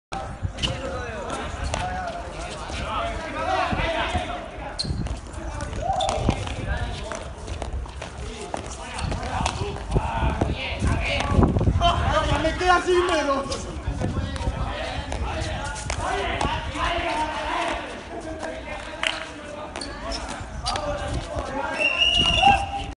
Clase de Pilota Valenciana
El alumnado de 1er curso del Ciclo Formativo de Grado Superior en Animación de Actividades Físicas y Deportivas (TAFAD) de Florida Universitària, recibe un curso de Pilota Valenciana en el Frontón de Catarroja.